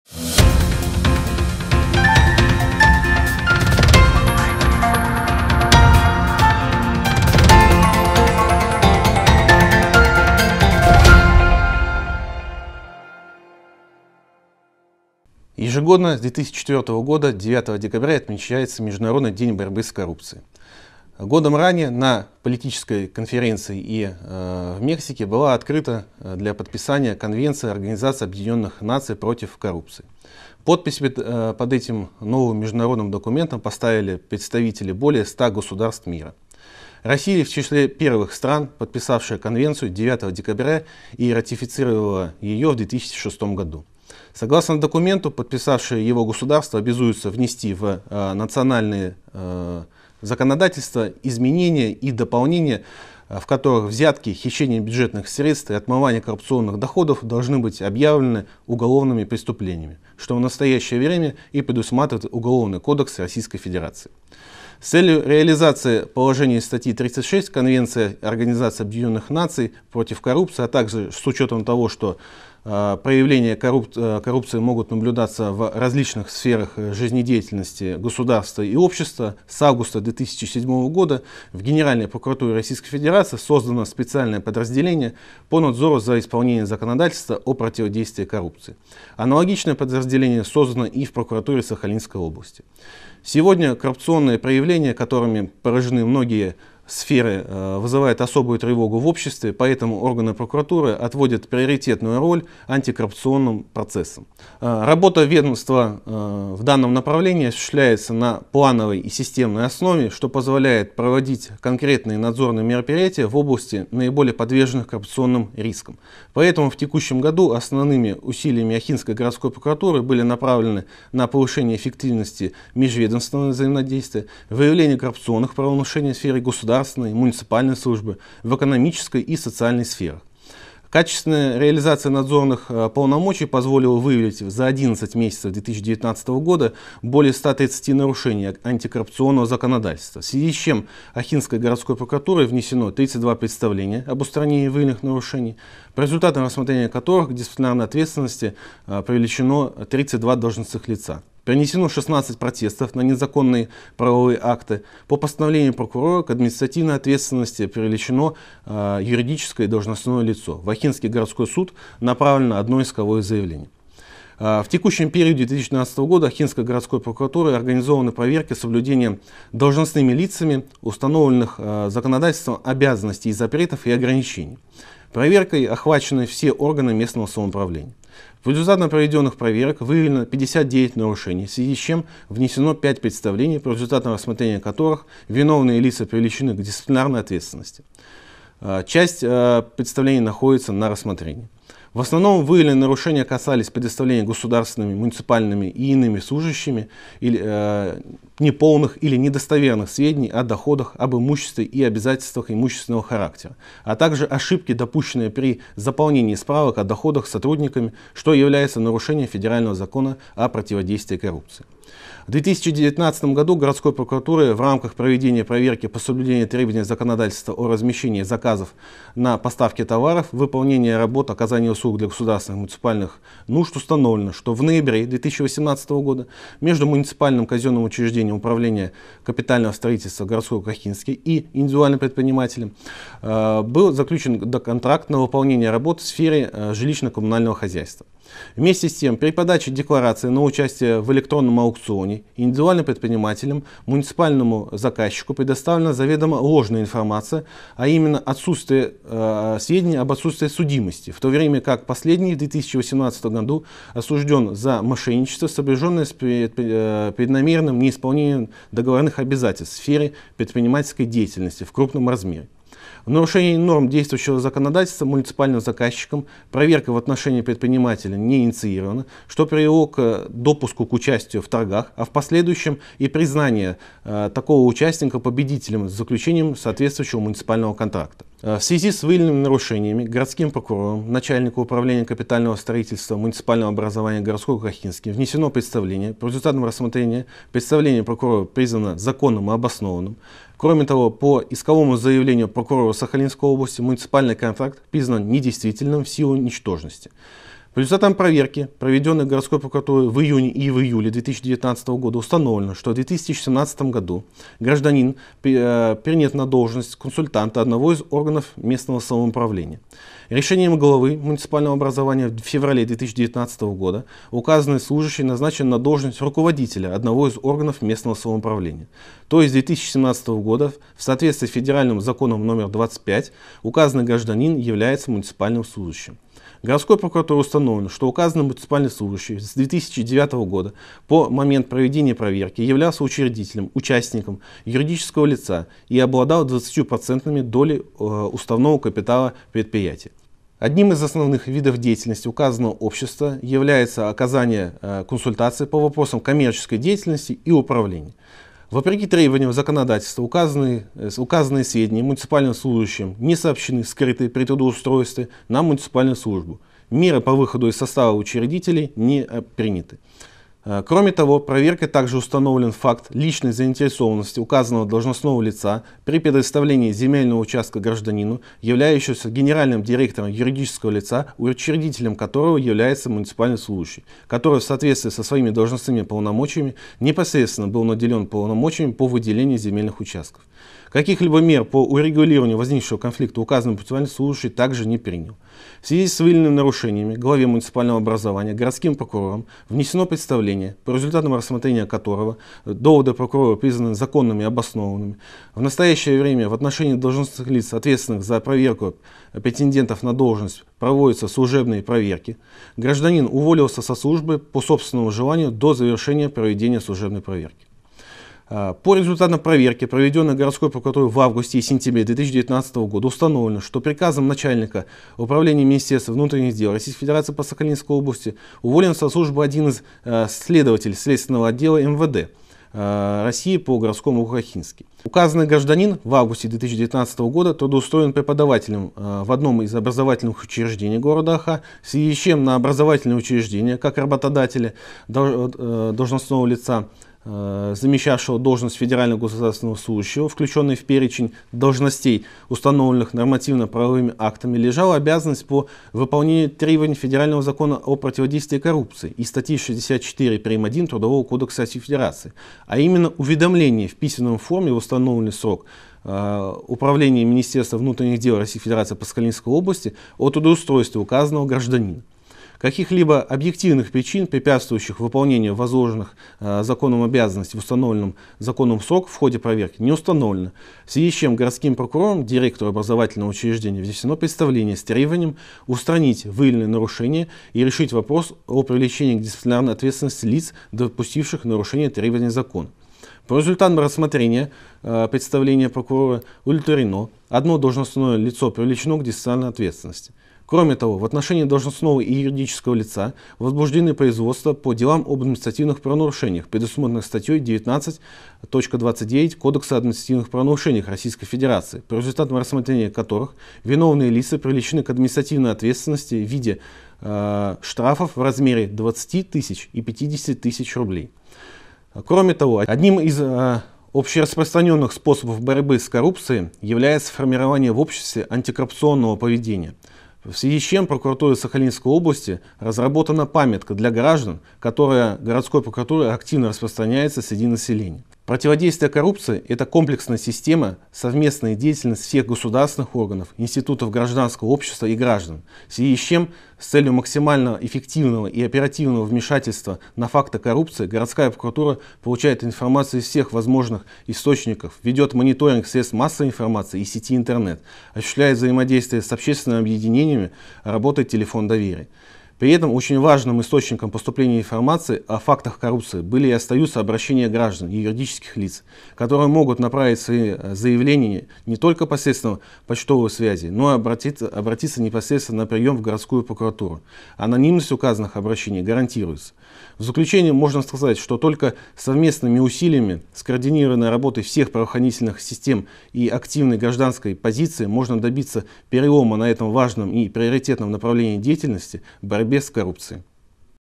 Интервью с Евгением Бутенко
9 декабря отмечается Международный день борьбы с коррупцией. В России Противодействие коррупции является общегосударственной задачей и ее решение возложено на все органы исполнительной, законодательной и судебной власти, а также на правоохранительные органы. В нашей студии и.о. городского прокурора Евгений Бутенко.